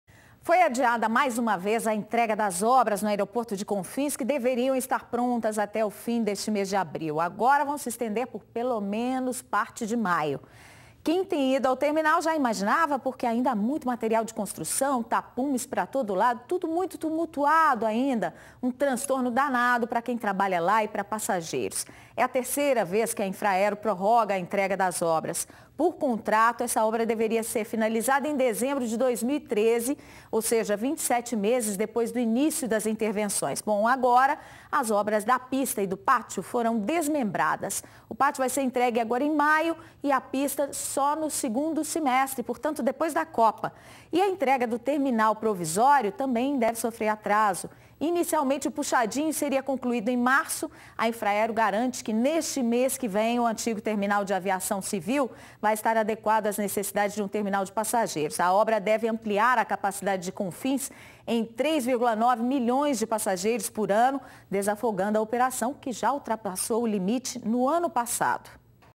Infraero atrasa, pela terceira vez, finalização de obras no aeroporto de Confins
Foi adiada, mais uma vez, a entrega das obras no aeroporto de Confins que deveriam estar prontas até o fim deste mês de abril. Agora, vão se estender pelo menos por parte de maio.